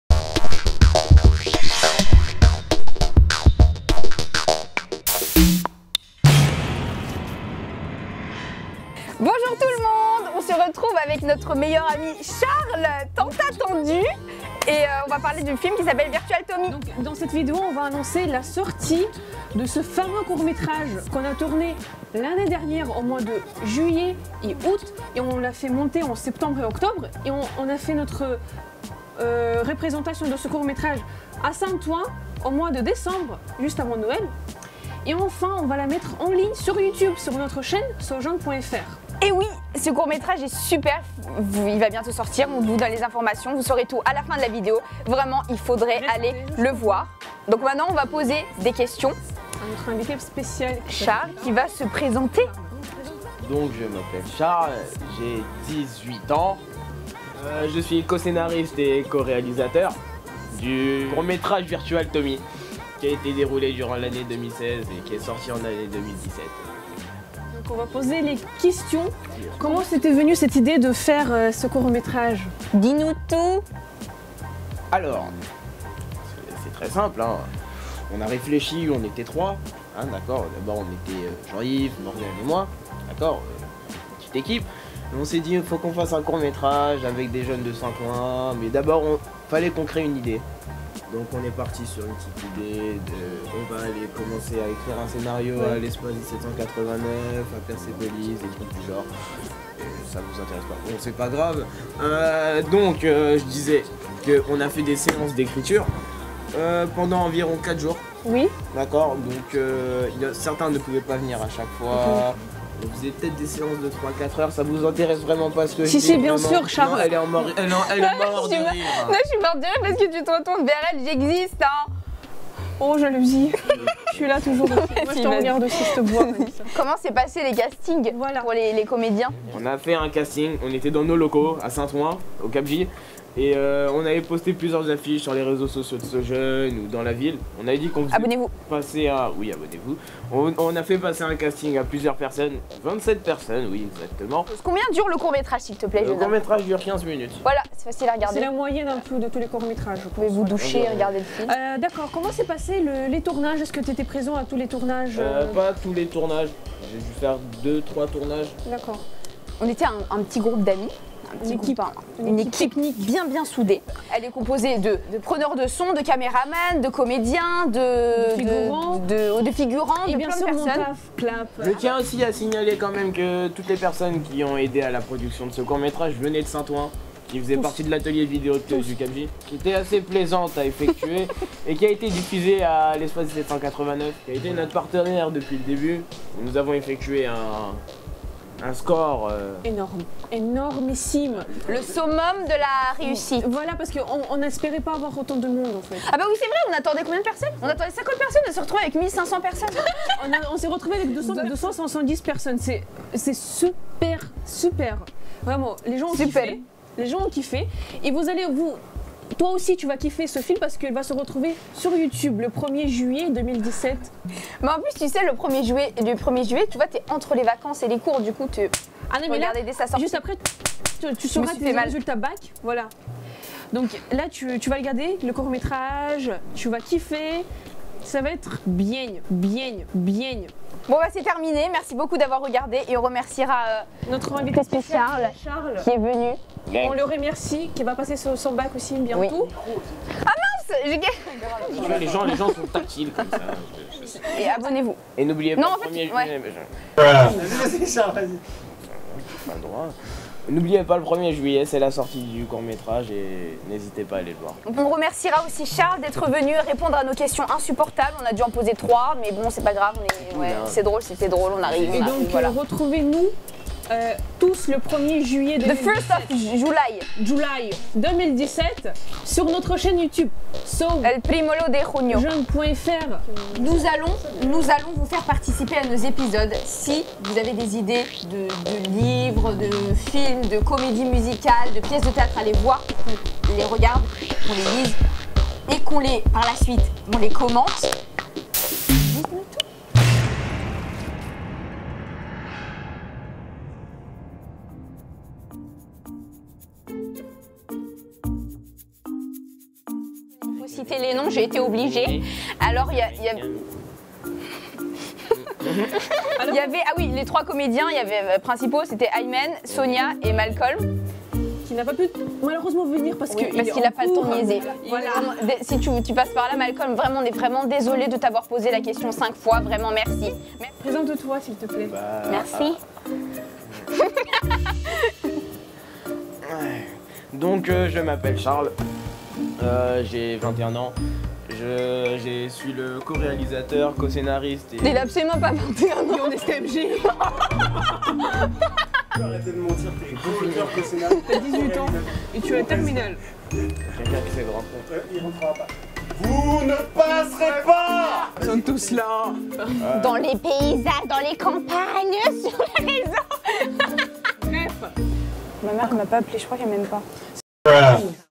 KHRYSTYSSA SHOW 5 Virtual Tomy
Le KHRYSTYSSA SHOW 5 est arrivé.
il nous annonce la sortie sur youtube du premier court métrage de l'atelier vidéo du cap'j, VIRTUAL TOMY.
C'est aussi le dernier numéro de la première saison,
à très bientôt pour la saison 2
N’hésitez pas à nous proposer des idées de sujets à commenter.
N'oubliez jamais qu'on vous aime, nos petits poussins au beurre salé.

KhryKhry et Nishaa